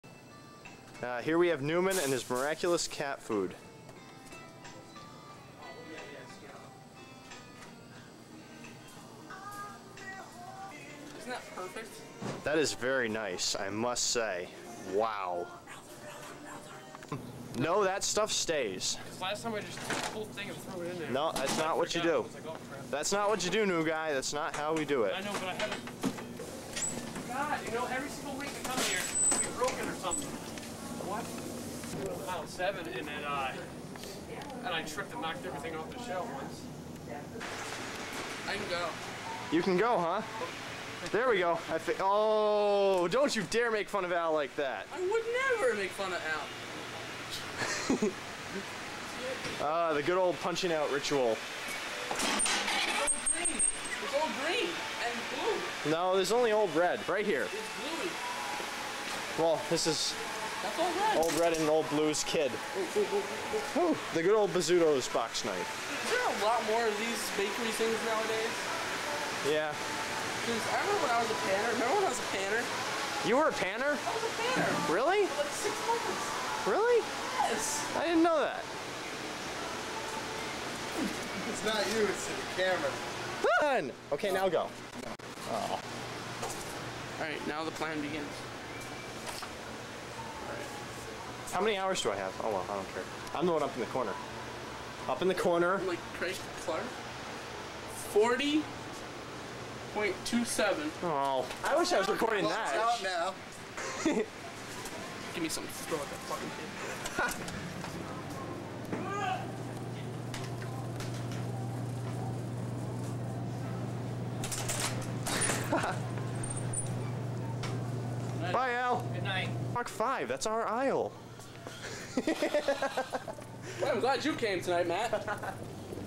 Jon Robert 2002
A random night in 2002 I annoyed Jon Robert with my video camera at Ellington Supermarket.